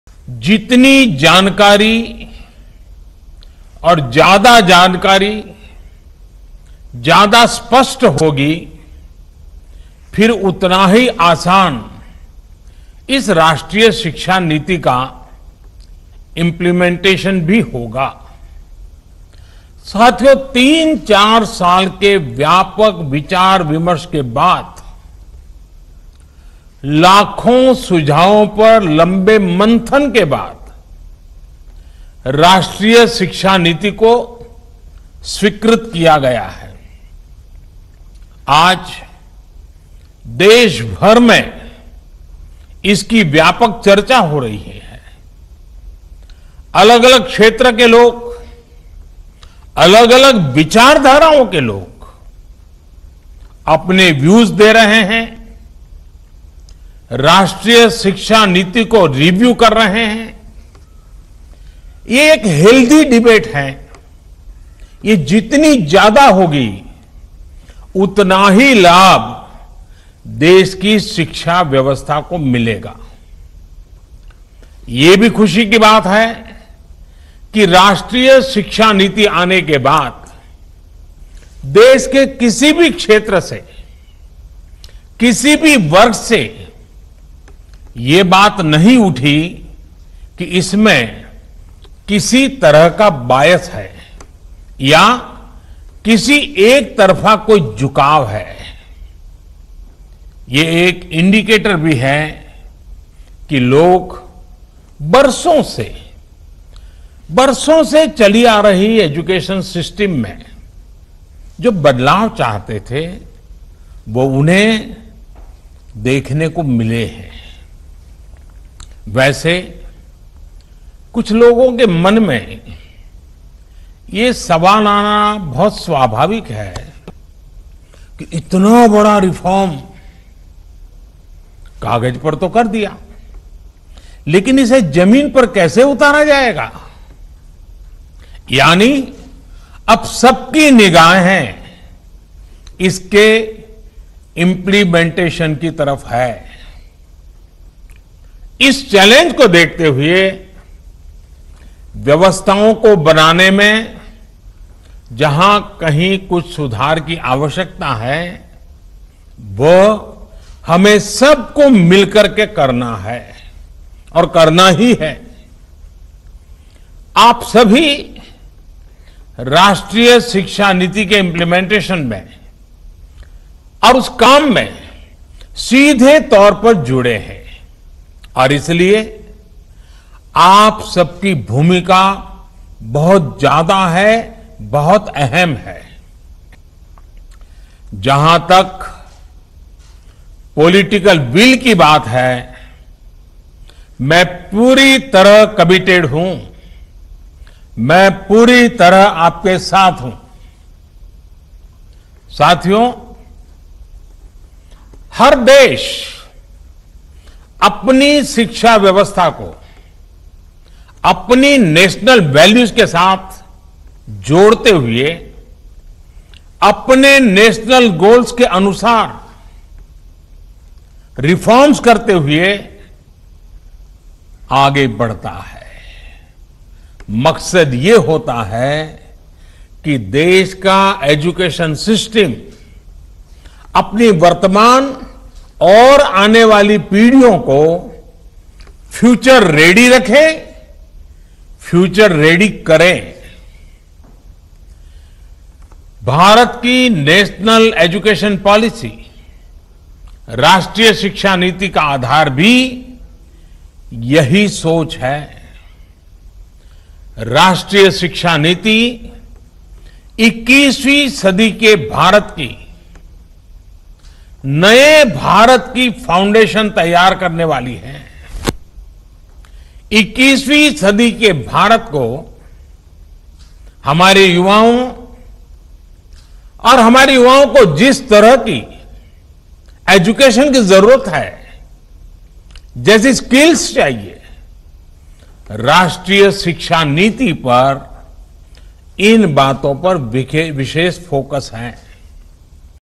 PM Narendra Modi: 'ಹೊಸ ಶಿಕ್ಷಣ ನೀತಿ ಬಗ್ಗೆ ವಿರೋಧ ಇಲ್ಲ, ಶಿಕ್ಷಣ ನೀತಿ ಬಗ್ಗೆ ಹೆಚ್ಚು ಮಾಹಿತಿ ತಿಳಿಯಬೇಕಿದೆ'
PM Narendra Modi:ಹೊಸ ರಾಷ್ಟೀಯ ಶಿಕ್ಷಣ ನೀತಿ ಕುರಿತು ಮಾತನಾಡಿದ ನರೇಂದ್ರ ಮೋದಿ, ಹೊಸ ಶಿಕ್ಷಣ  ನೀತಿಗೆ ವಿರೋಧ ಇಲ್ಲ, ಶಿಕ್ಷಣ ನೀತಿ ಬಗ್ಗೆ ಹೆಚ್ಚು ತಿಳಿಯಬೇಕಿದೆ ಎಂದು ಹೇಳಿದ್ದಾರೆ.